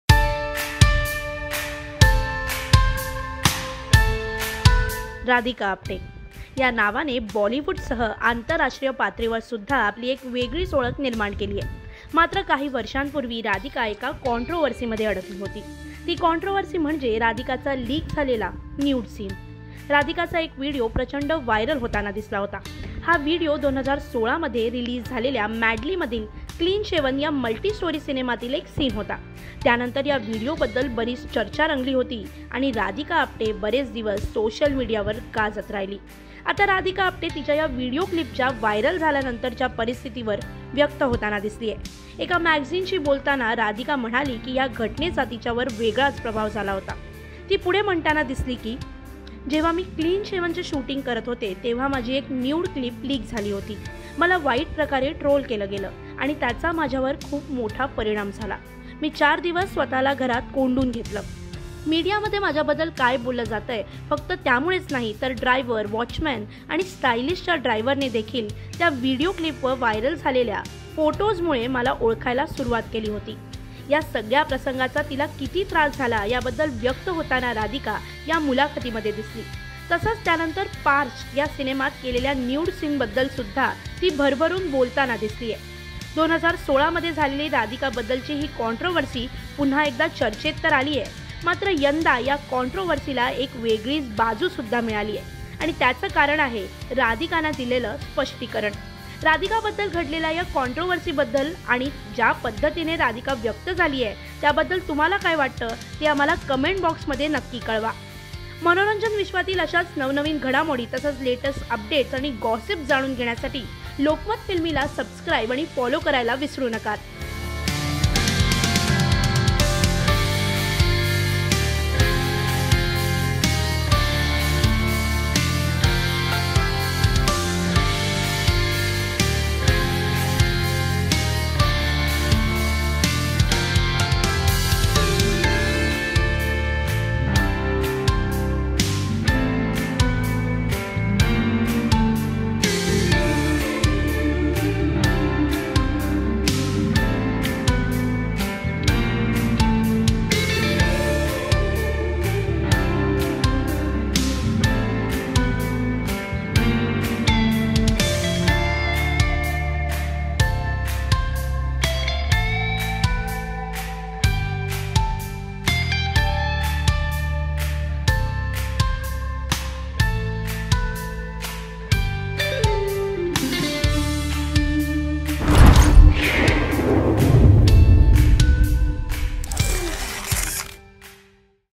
न्यूड सीन बद्दल Radhika Apte झाली व्यक्त | Radhika Apte Nude Scene | Lokmat Filmy
राधिका आपटे या नावाने बॉलिवूडसह आंतरराष्ट्रीय पातळीवर सुद्धा आपली एक वेगळी ओळख निर्माण केली आहे मात्र काही वर्षांपूर्वी राधिका एका कॉन्ट्रोव्हर्सी मध्ये अडकली होती ती कॉन्ट्रोव्हर्सी म्हणजे राधिका चा झालेला न्यूड सीन... राधिका चा एक व्हिडिओ व्हायरल झाला होता...हा व्हिडिओ 2016मध्ये प्रदर्शित झालेल्या 'मॅडली’मधील ‘क्लीन शेवन' या मल्टी स्टोरी सिनेमातील एक सीन होता... त्यानंतर या व्हिडीओ बद्दल बरीच चर्चा रंगली होती आणि राधिका आपटे बरेच दिवस सोशल मीडियावर गाजत राहिली... आता राधिका आपटे तिच्या या व्हिडीओ क्लिपच्या व्हायरल झाल्यानंतरच्या परिस्थितीवर व्यक्त झाली आहे.